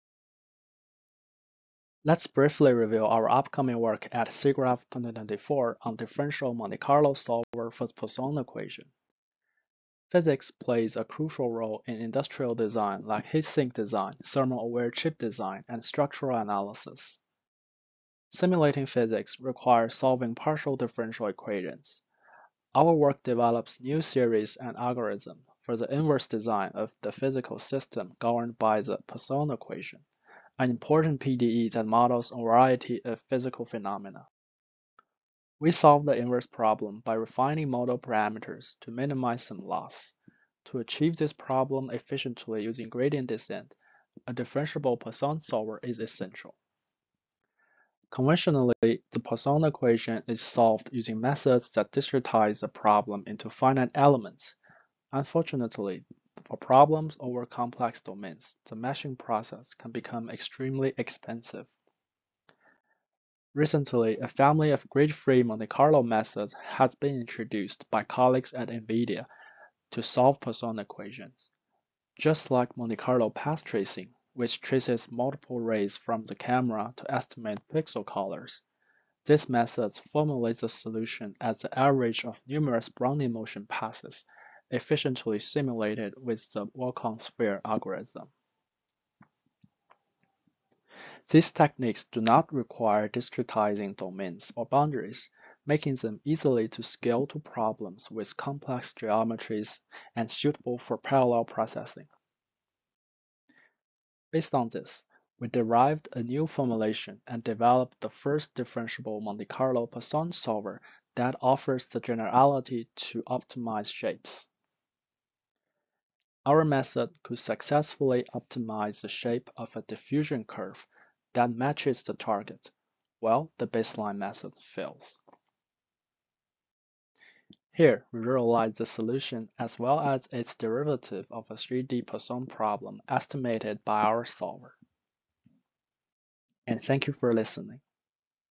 A Differential Monte Carlo Solver For the Poisson Equation | NVIDIA Research
The Poisson equation is an important partial differential equation (PDE) with numerous applications in physics, engineering, and computer graphics. Conventional solutions to the Poisson equation require discretizing the domain or its boundary, which can be very expensive for domains with detailed geometries. To overcome this challenge, a family of grid-free Monte Carlo solutions has recently been developed. By utilizing walk-on-sphere (WoS) processes, these techniques are capable of efficiently solving the Poisson equation over complex domains.

In this paper, we introduce a general technique that differentiates solutions to the Poisson equation with Dirichlet boundary conditions. Specifically, we devise a new boundary-integral formulation for the derivatives with respect to arbitrary parameters including shapes of the domain. Further, we develop an efficient walk-on-spheres technique based on our new formulation---including a new approach to estimate normal derivatives of the solution field. We demonstrate the effectiveness of our technique over baseline methods using several synthetic examples.